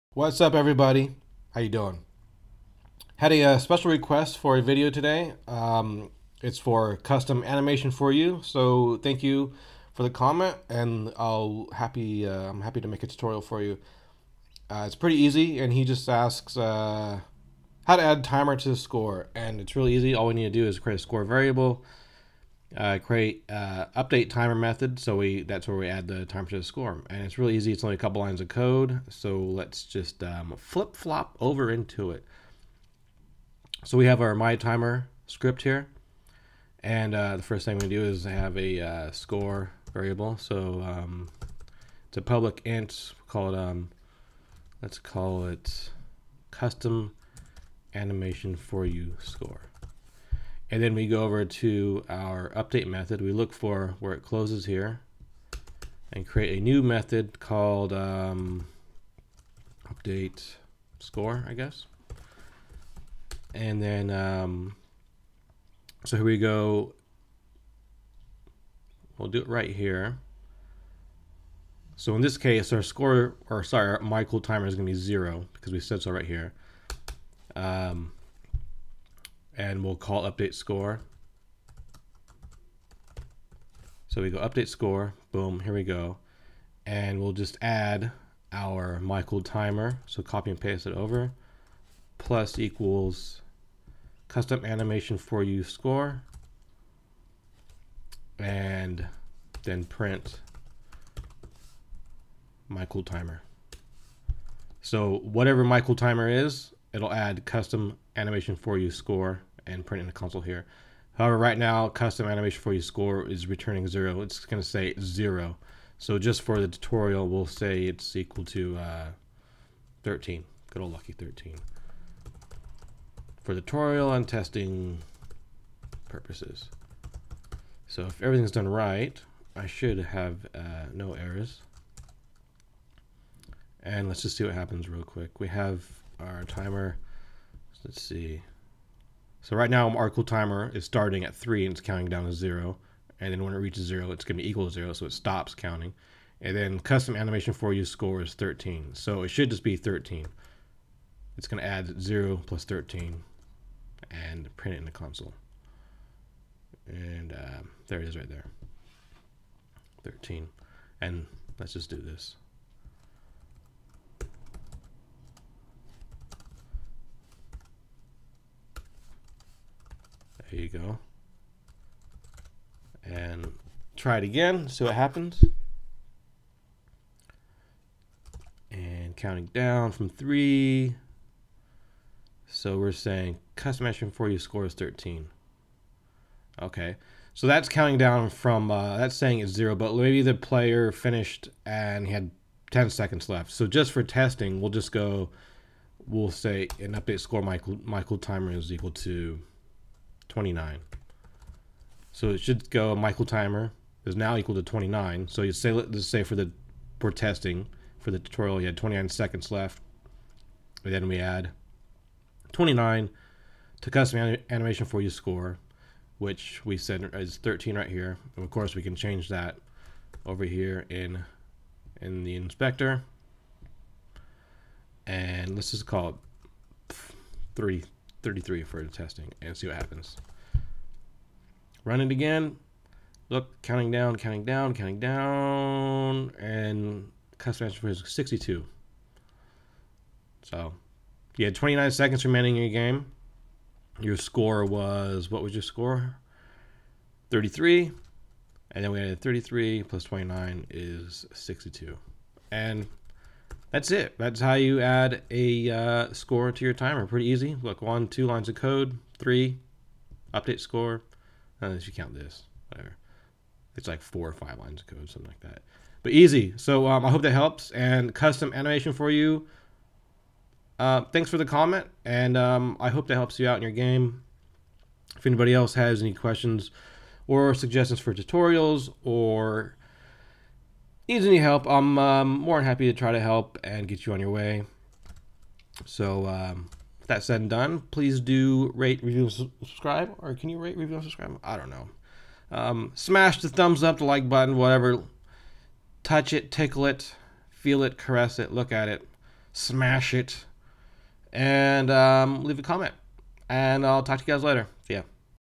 How to Add Countdown Timer to Score in Unity 5 Tutorial
How to Add Timer to Score

* Create Score Variable
* Create Update Timer Method
* Add the Timer to the Score

Tutorial Video for: customanimation4u

All we need to do is add our myCoolTimer to our score variable,
so, let’s make one.

After that, let’s create a new method and add the customanimation4uScore to our myCoolTimer variable.

Next, print the results in the console.

Play test and enjoy your game!

If this video helped you, please give the video a big thumbs up, I appreciate it.